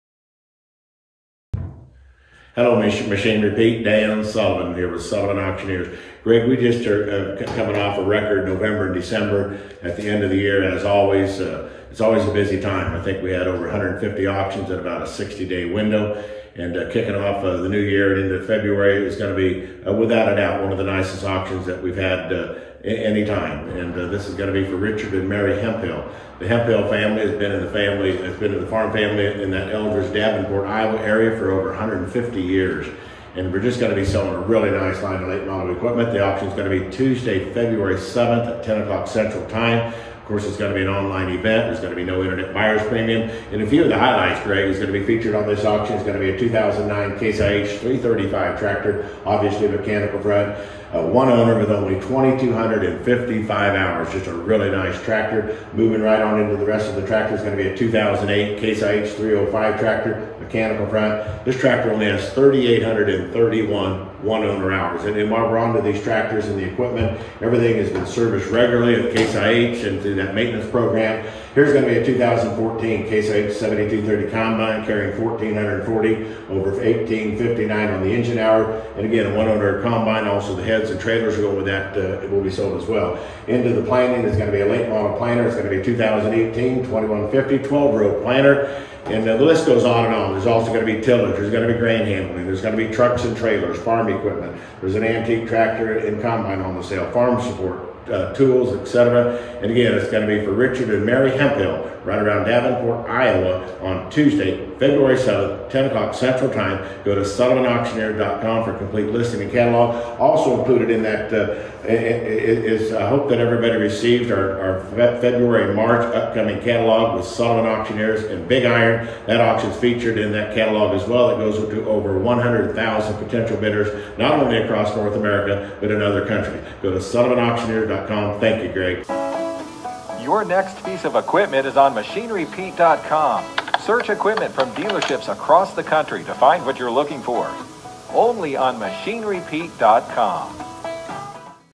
Preview of Richard and Mary Hemphill Online Farm Retirement Auction 2/7/23 in Davenport, IA
Dan Sullivan with Sullivan Auctioneers gives us a video preview of the Richard and Mary Hemphill online farm retirement auction Tuesday, Feb. 7 in Davenport, IA. Super sharp "1 owner" line of equipment. 2009 CaseIH Magnum 335 with 2255 hours; 2008 CaseIH Magnum 305 with 3831 hours; 2014 CaseIH 7230 combine with 1440 sep. hours; 2018 CaseIH 2150 12-row planter. Full line of equipment for sale.